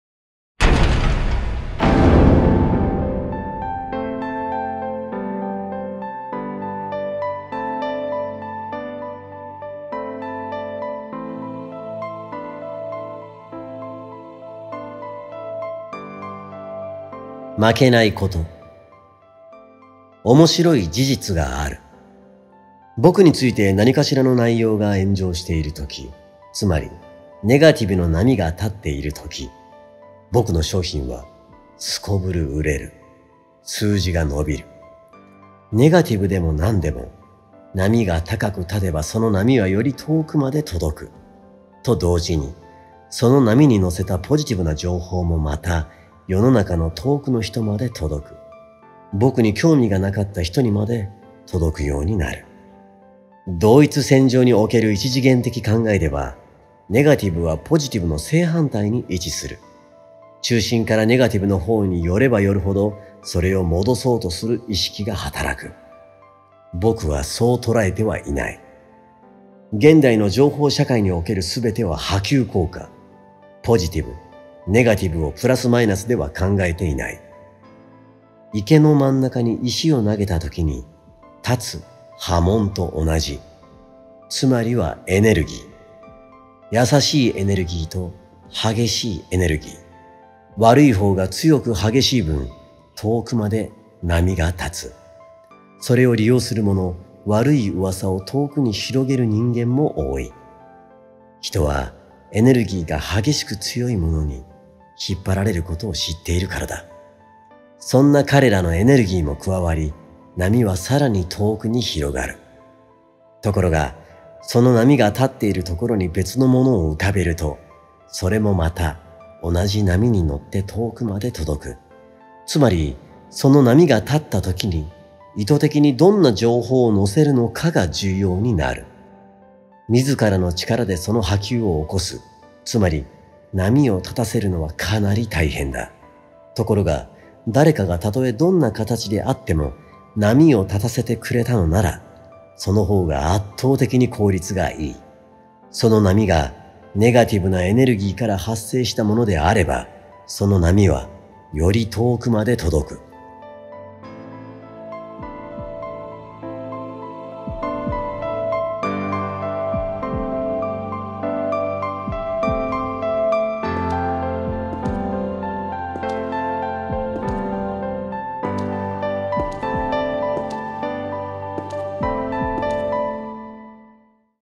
GACKTの勝ち方特装版朗読CD試聴動画vol.098「GACKTが、炎上しても売れる理由」
「GACKTの勝ち方特装版朗読CD3枚組付」が特設販売サイトにて販売中！